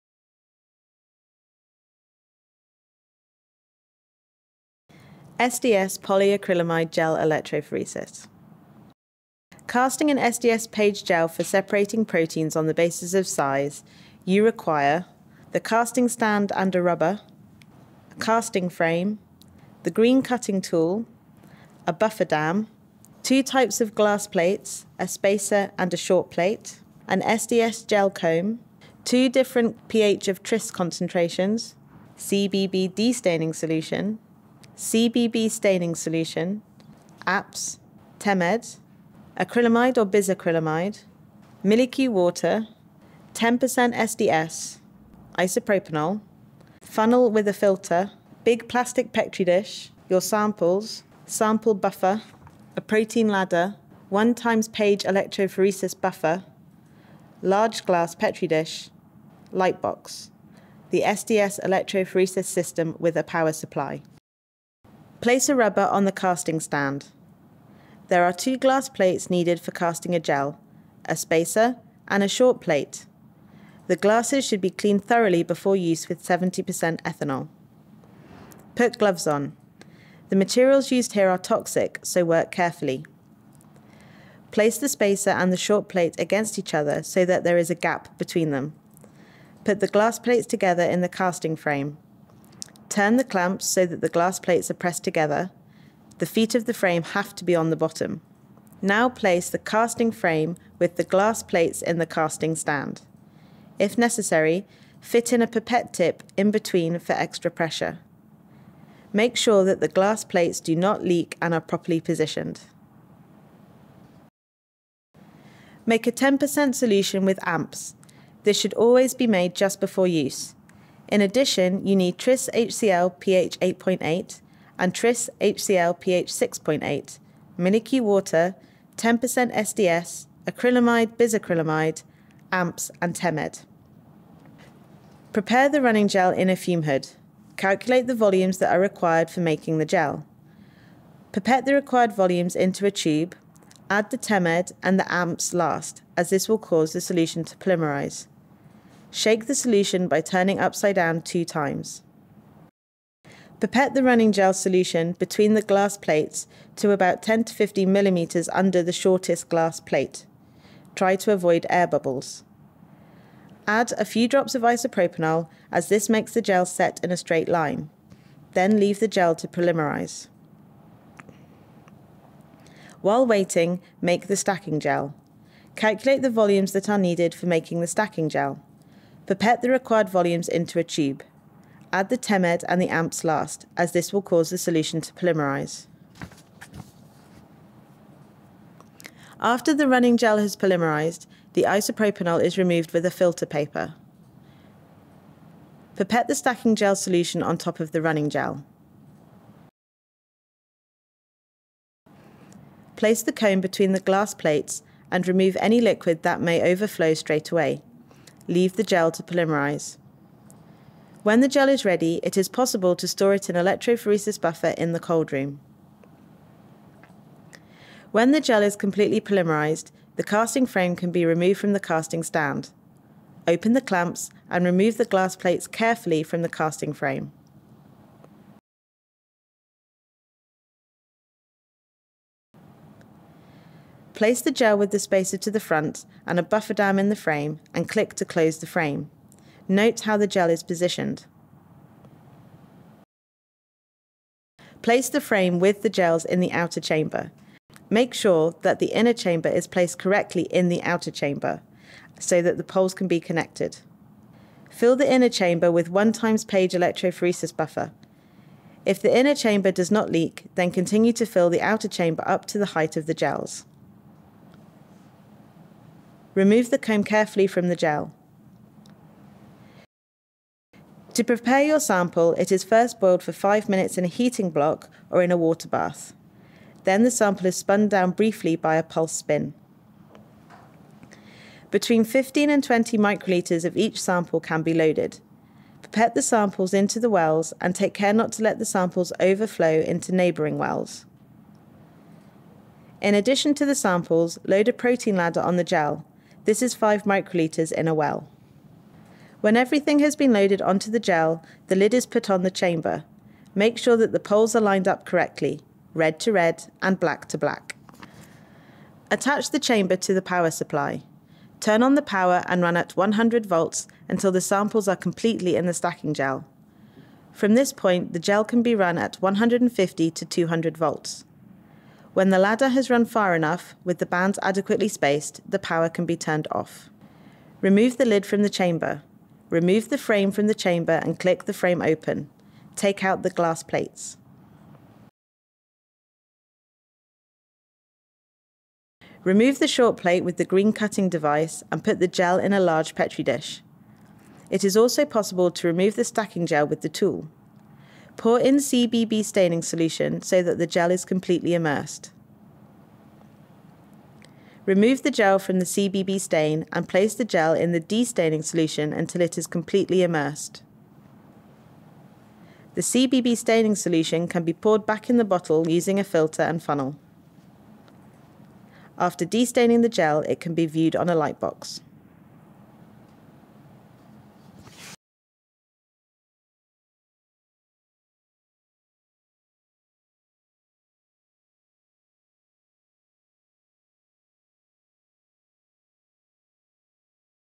HAN Bachelor Courses | Life Sciences | SDS-PAGE
In this video you see how to execute a SDS-PAGE.